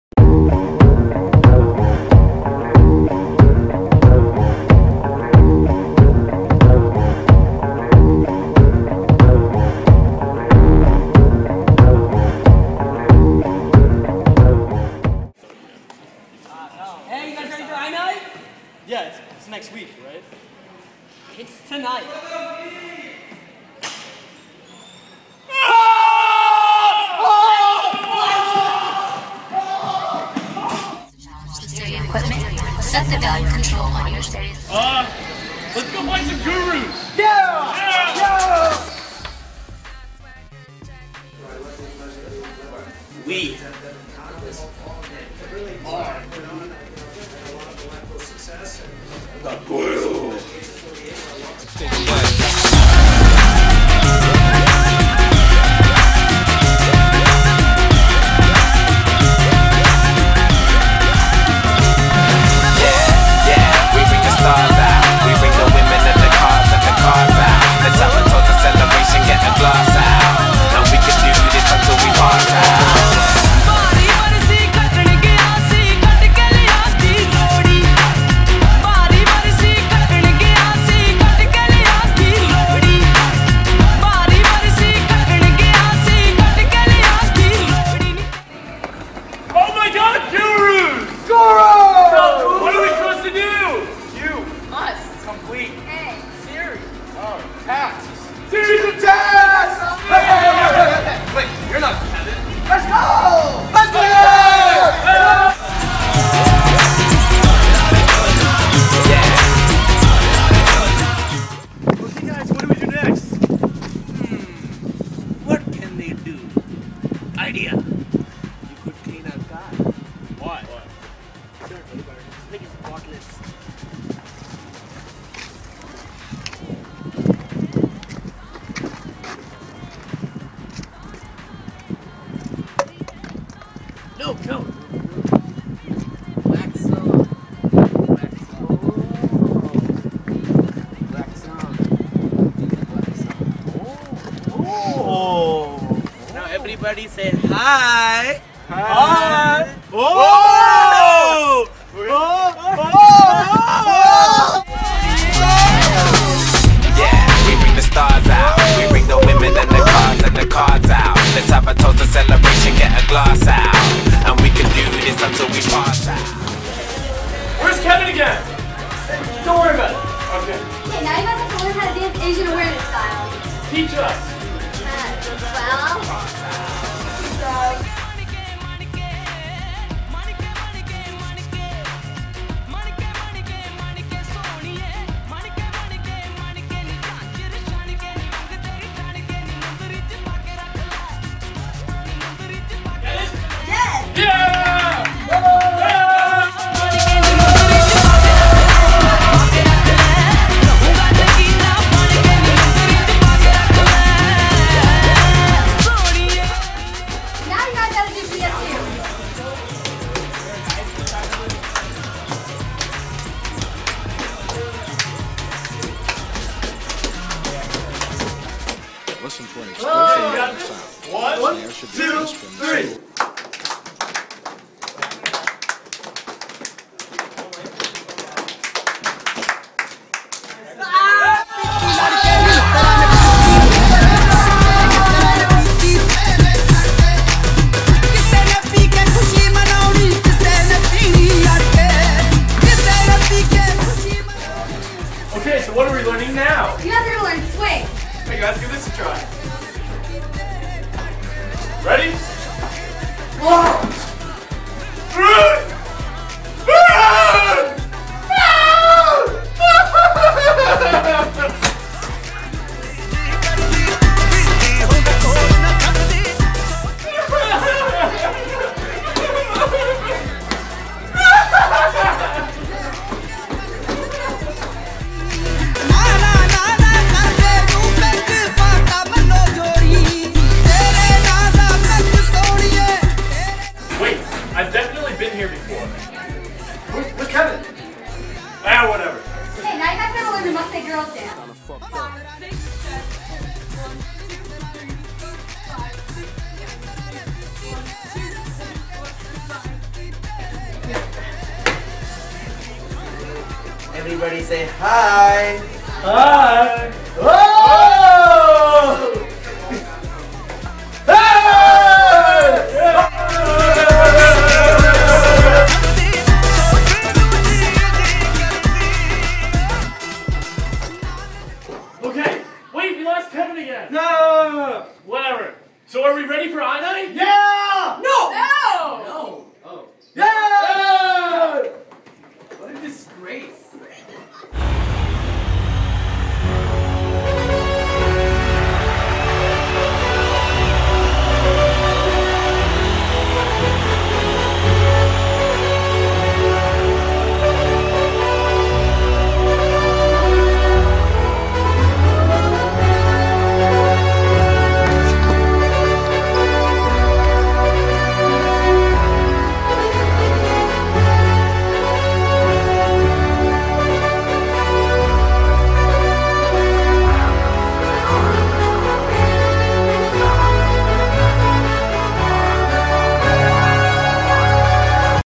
TJHSST I-Nite 2012 Intro
Kevin, Wills, and Varun forget to prepare for I-Nite and must scramble to save it.

Cast: Kevin, Wills, Varun, and the Namaste Officers, with appearances from various acts.

Video by Mayank Jain